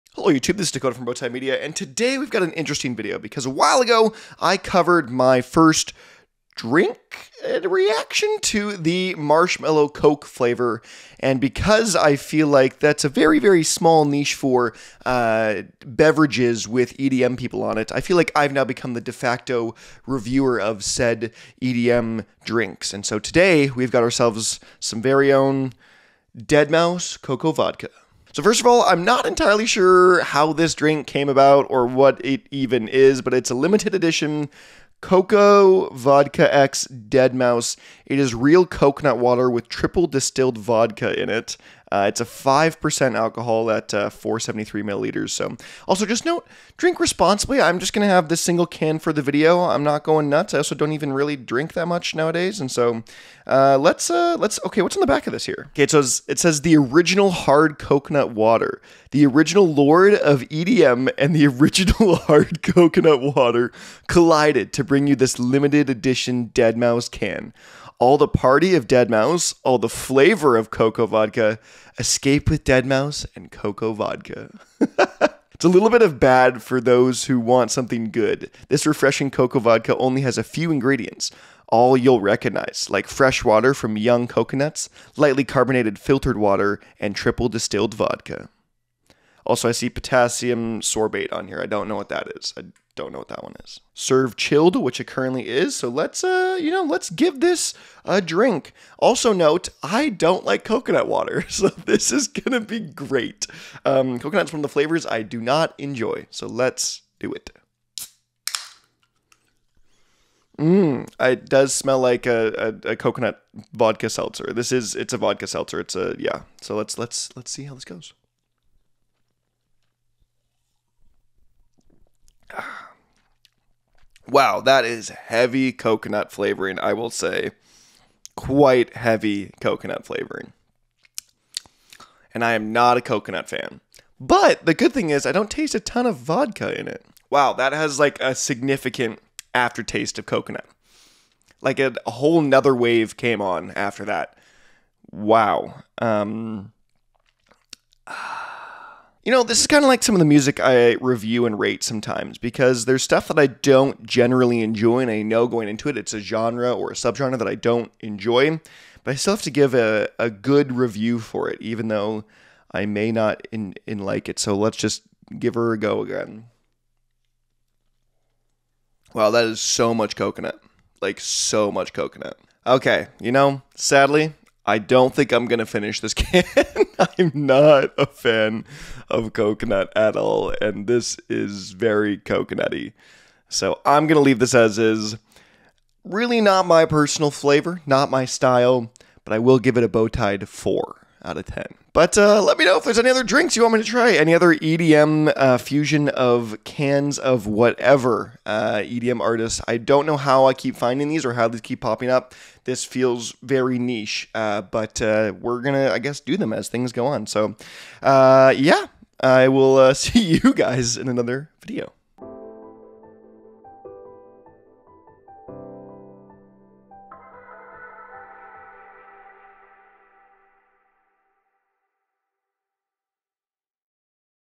I Tried "deadmau5 Vodka" So You Don’t Have To
It appears that deadmau5 and Coco Vodka have teamed up for a collaboration in the style of refreshments. As the new and only EDM-drink reviewer, let's see how this tastes.